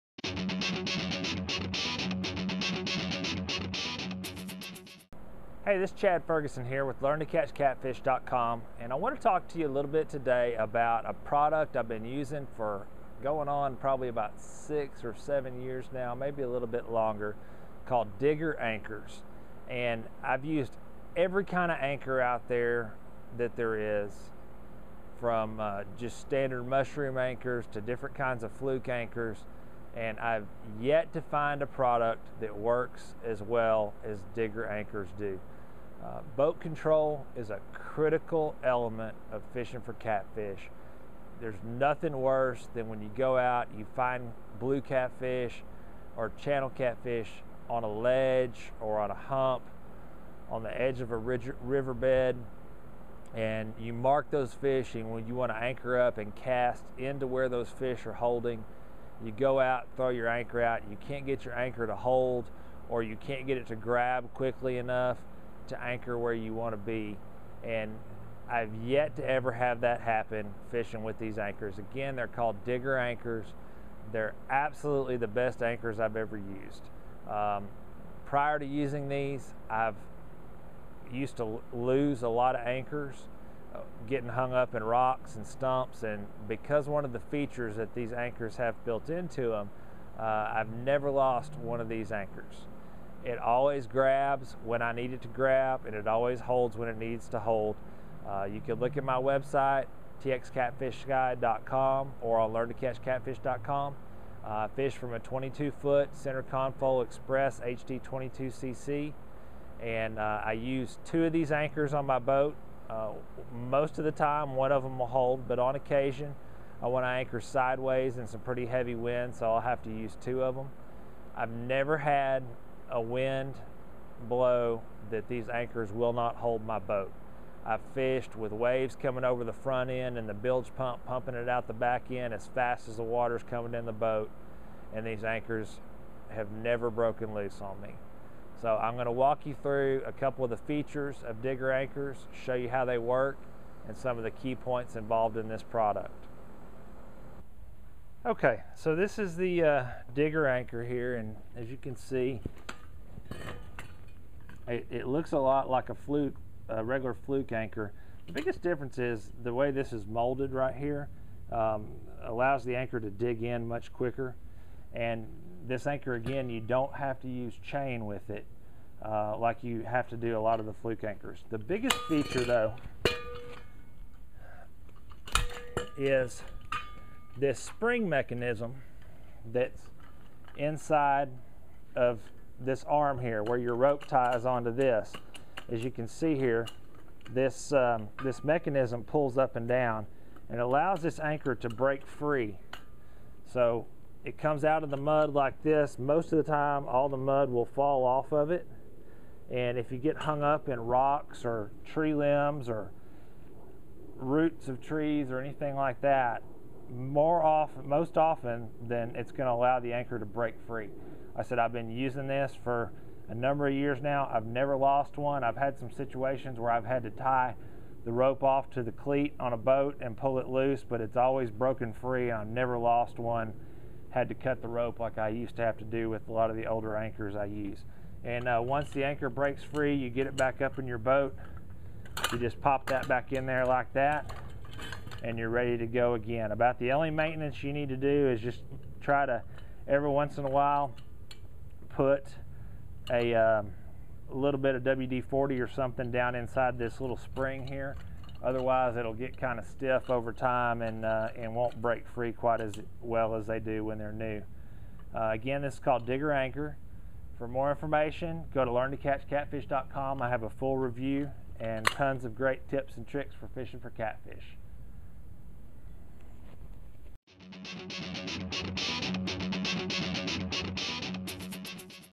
Digger Anchor Product Review
- Digger Anchor is a product that I have been using for many years and have been very successful with. The Digger Anchor is hands down the absolute very best boat anchors that I have ever used.